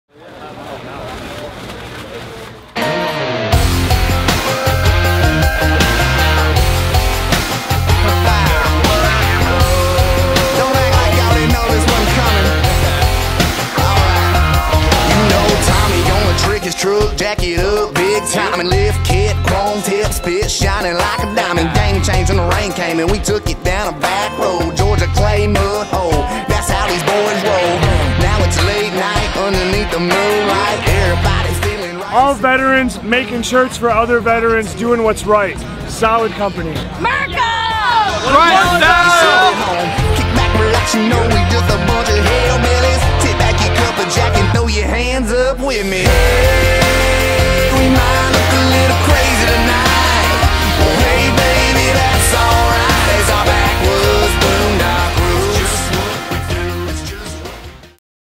More Grunt Style at Country Thunder
Here's another look at the awesome time we had at Country Thunder, selling shirts and spreading freedom!